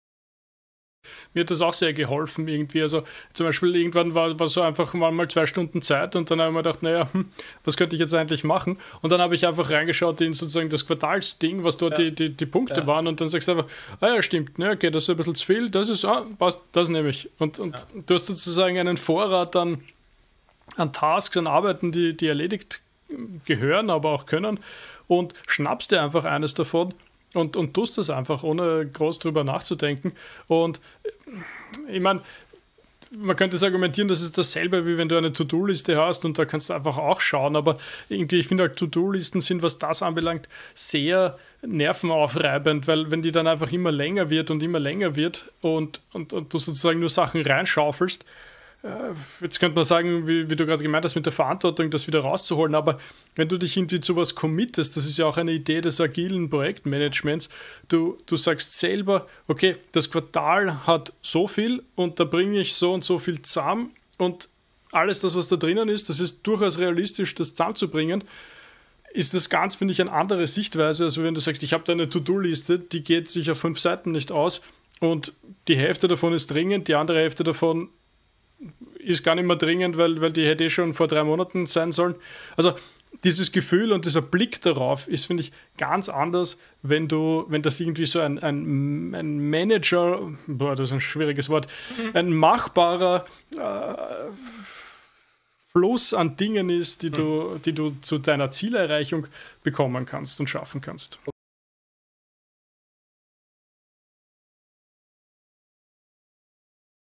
Was ist der Unterschied zwischen einem Quartalsplan und einer To-Do-Liste?
In diesem kurzen Clip spricht Flo über die Nutzung eines Quartalsplan im Arbeitsalltag statt einer einfachen To-Do-Liste.